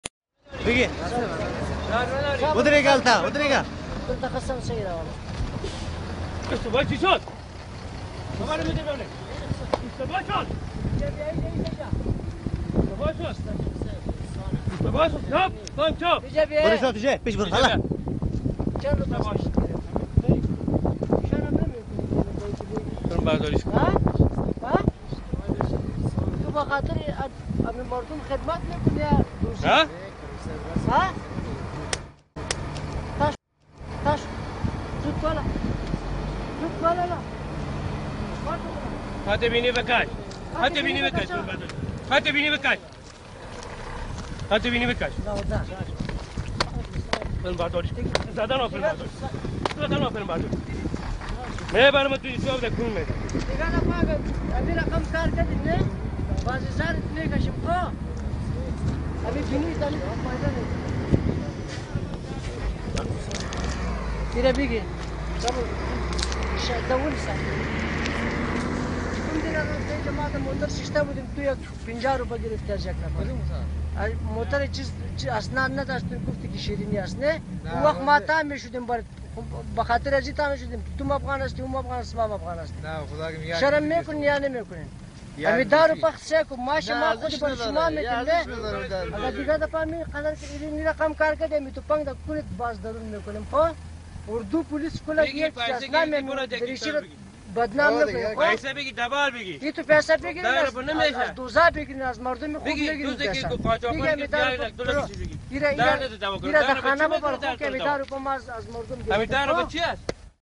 Afghanistan police ( Tajik ) taking Money from passenger !! 2012
jalalabad pashtoon men is beating police ..that why u taking Money !! from people !! farsiwan losser !!